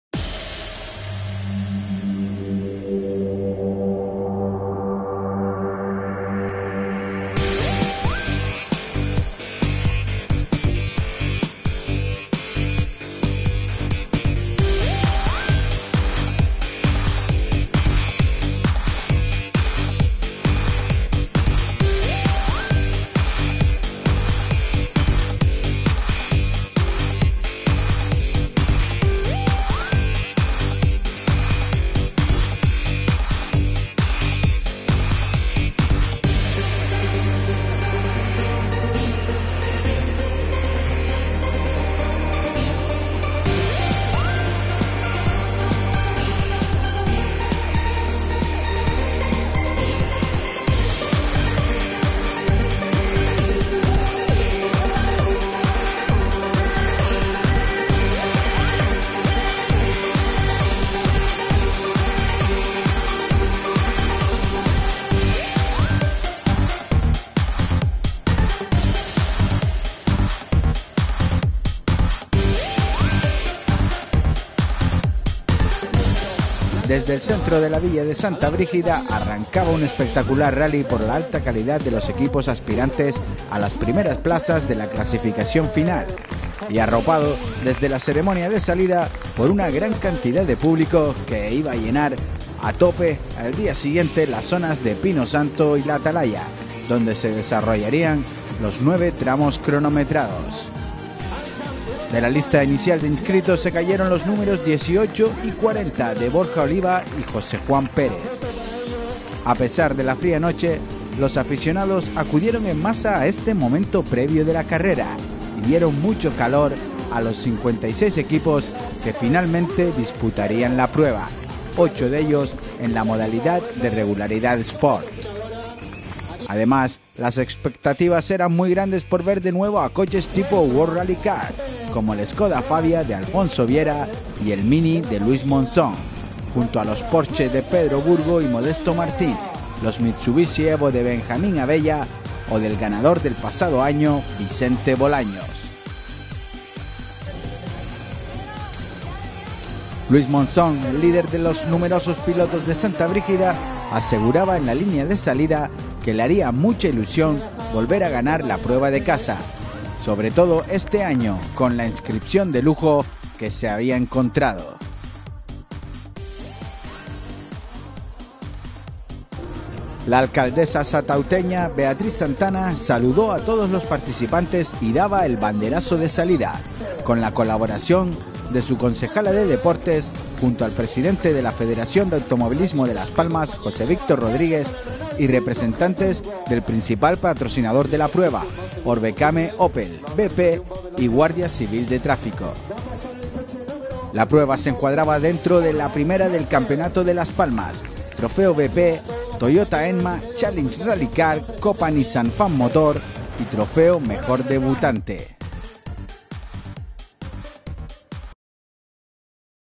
31 Rally Santa Brígida 2015 - Salida
ARRANCÓ UN ESPECTACULAR RALLYE DE SANTA BRÍGIDA CON MUCHO PÚBLICO DESDE LA SALIDA
A pesar de la fría noche, los aficionados acudieron en masa a este momento previo de la carrera y dieron mucho “calor” a los 56 equipos que finalmente disputarían la prueba, ocho de ellos en la modalidad de Regularidad Sport.
 Además, las expectativas eran muy grandes por ver de nuevo a coches tipo WRC como el Skoda Fabia de Alfonso Viera y el MINI de Luis Monzón, junto a los Porsche de Pedro Burgo y Modesto Martín, los Mitsubishi Evo de Benjamín Avella o del ganador del pasado año, Vicente Bolaños.